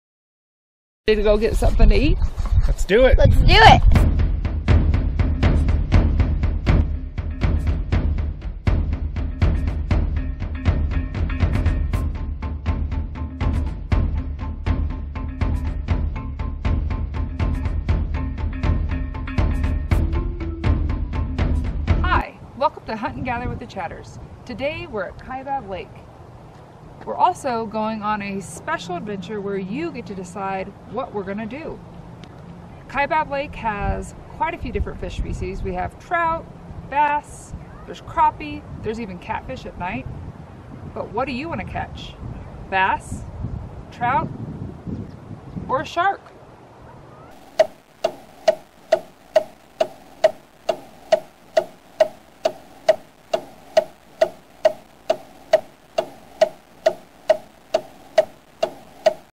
KAIBAB LAKE FISHING - INTERACTIVE - Trout - Bass - Boat - Shore - 3 Species - 9 Methods
The Chatters family take you to Kaibab Lake, where you get to CALL ALL THE SHOTS! shortvideo .Choose between 3 different species, 9 different methods (lures and baits), to catch a total of 40 FISH! Try all the choices to learn the most about this lake whether fishing for trout, bass, shore, boat, lures or bait! TRY IT, YOU'LL LOVE IT! You can even keep a nice fish and learn a cool recipe.